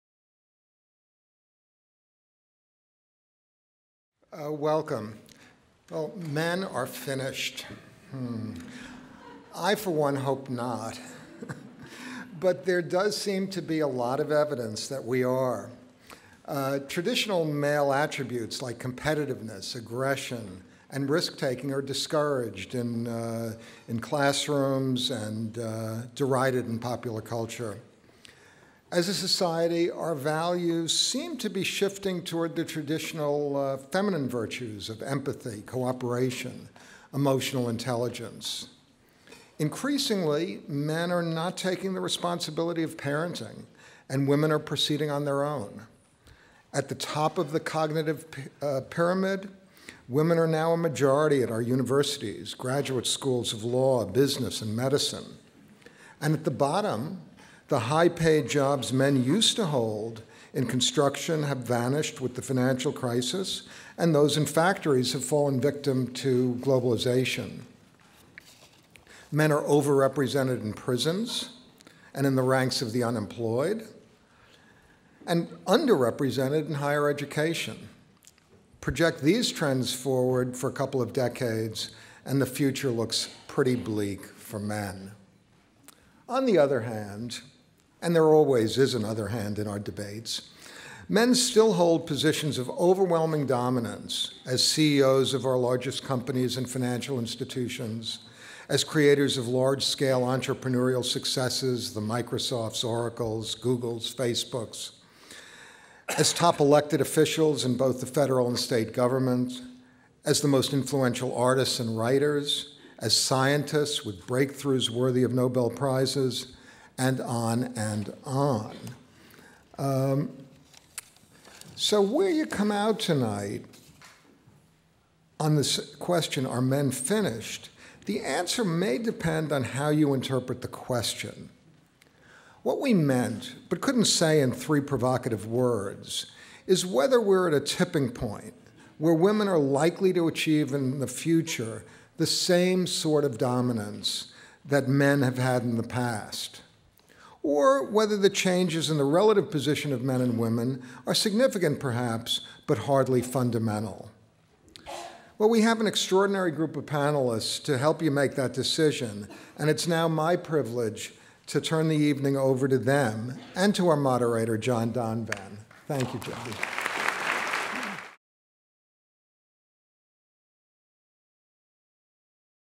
Debate: "Men Are Finished" Robert Rosenkranz Introduction
Robert Rosenkranz, Intelligence Squared U.S. Chairman, and moderator John Donvan discuss the debate "Men Are Finished."

About the debate, from Intelligence Squared U.S.:
In a modern, post-industrial economy that seems better suited to women than men, many are wondering if men have been permanently left behind. Education and employment statistics point to a clear and growing dominance in women’s status at home and in the workplace. Are men primed for a comeback or have the old rules changed for good?

For the Motion:
Dan Abrams,
ABC News Legal Analyst & Author of Man Down

Hannah Rosin,
Award-Winning Journalist for Slate and The Atlantic

Against the Motion:
Christina Hoff Sommers,
Feminist Scholar & Author of The War Against Boys

David Zinczenko,
Editor-in-Chief of Men’s Health Magazine

Moderator:
John Donvan
Author & Correspondent for ABC News